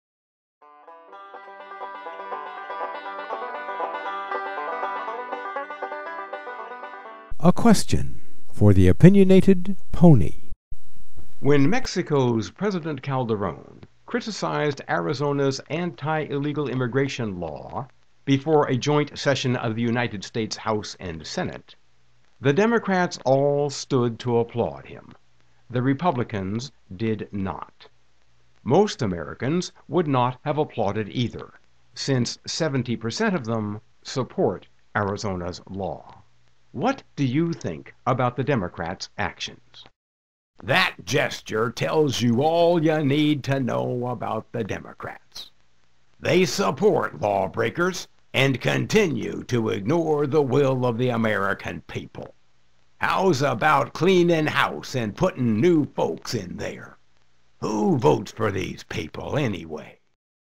All You Need To Know About The Democrats
The Opinionated Pony has a few words to say about what we learned about the Democrats when they applauded Mexican President Calderon's condemnation of Arizona's Anti-Illegal-Immigration law.  The Democrats obviously don't agree with the Arizona approach, while 70% of Americans support it.  Looks like the Democrats are a little out of touch with the voters.